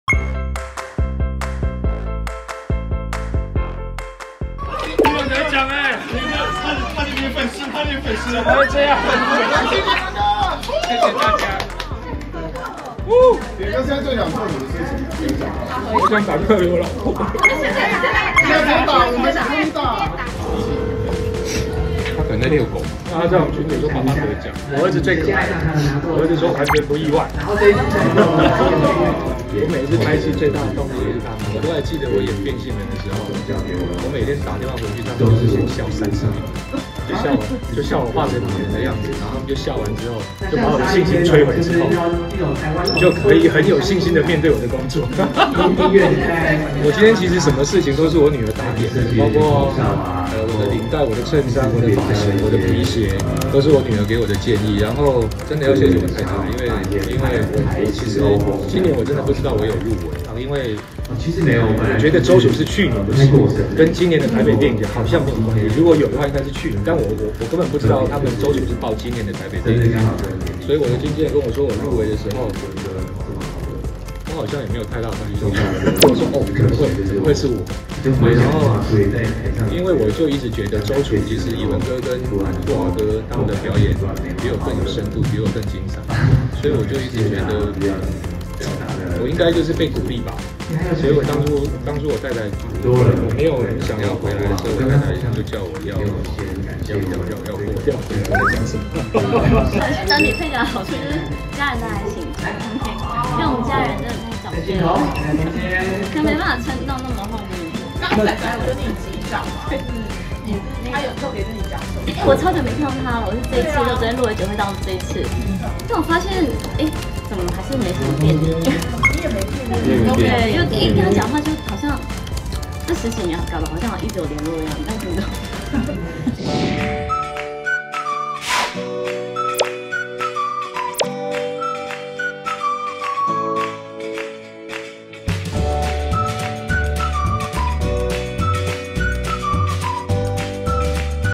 【撩星聞】李李仁奪獎嗨到不知自己在講什麼 陳意涵擊掌周渝民：好像一直有聯絡XD｜台北電影節
李李仁與陳意涵喜奪本屆台北電影節最佳男配角與最佳女配角。